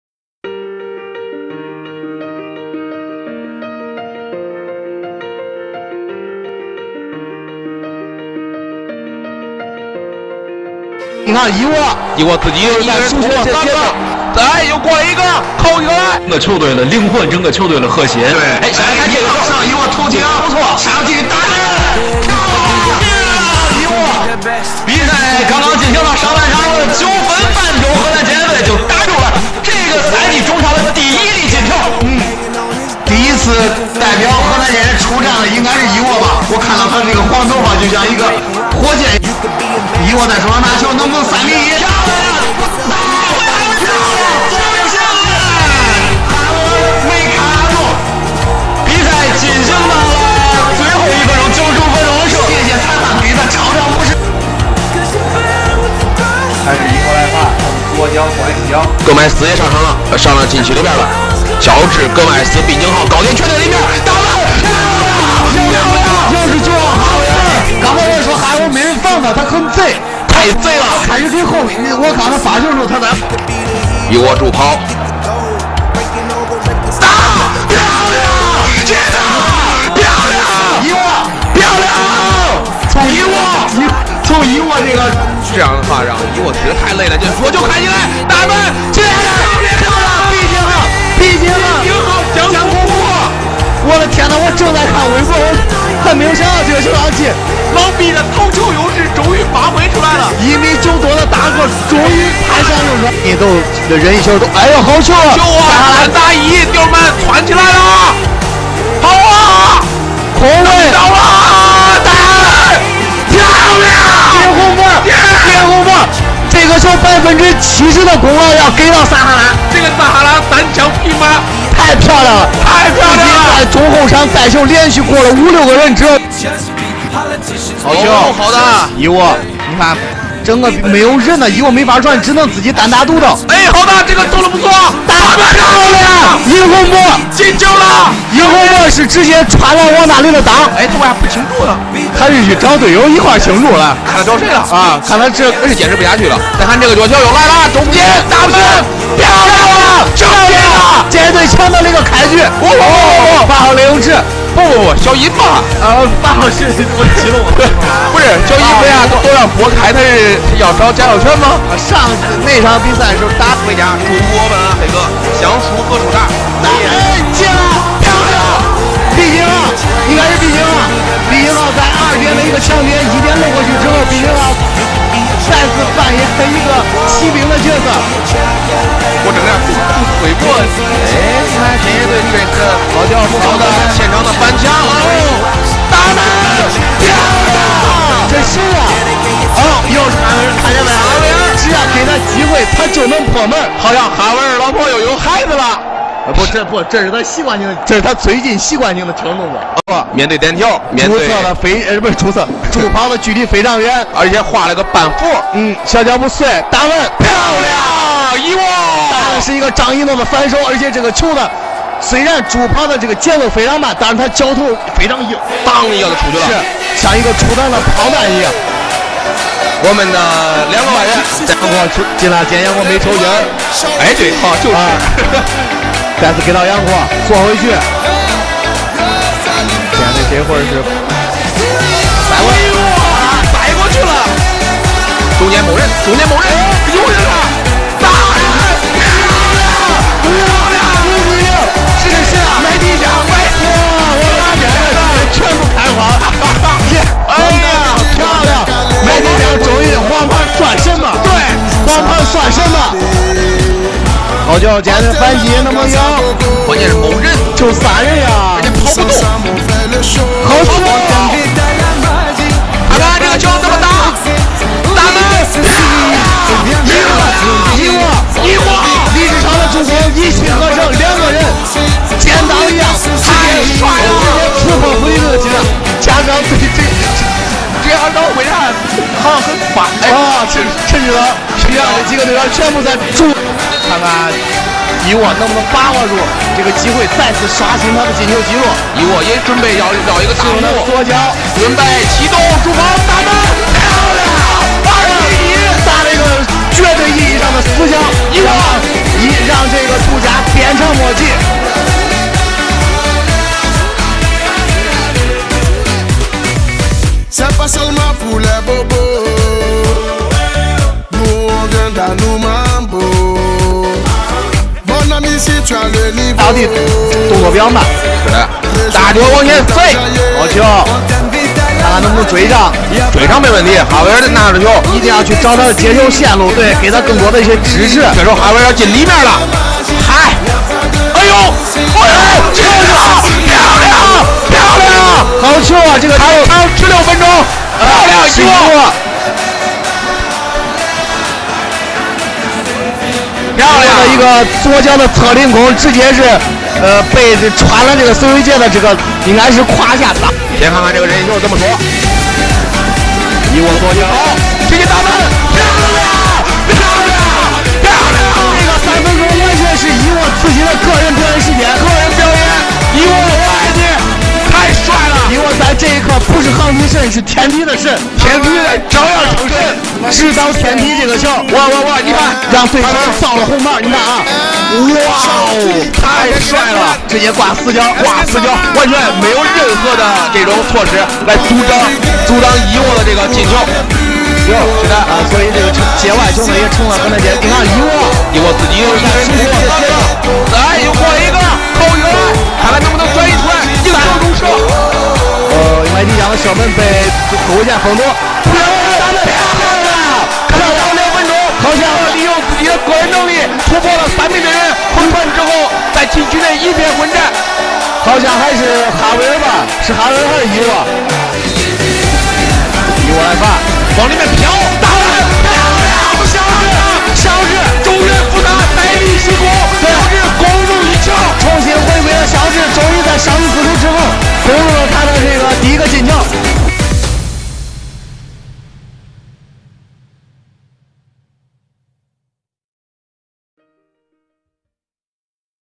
Highlight of Olivio Da Rosa in 2015
Ivo, the assists title of China Super League, scored 7 goals and assisted 11 times, helped Henan Jianye FC gott a remarkable achievement in 2015.